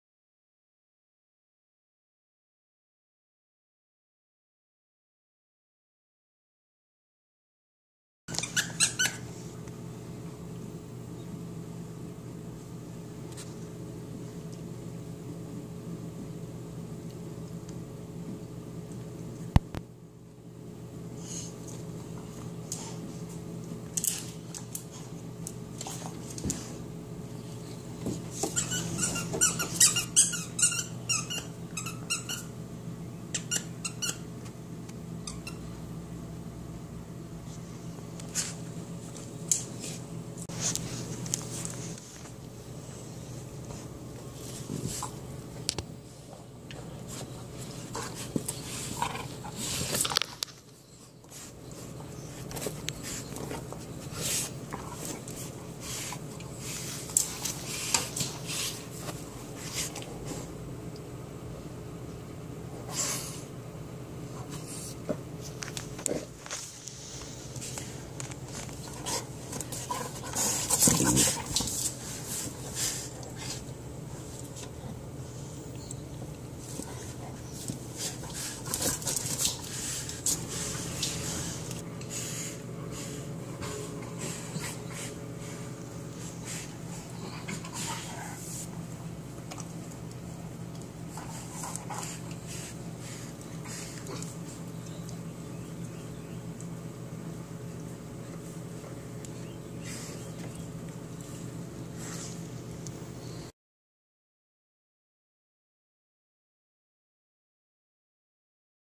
Play With Me
Riley and Madison take a break from watching the bunnies and running in the field. They decide to come inside to nap, but Madison has other ideas...        A BunnyNature.org Production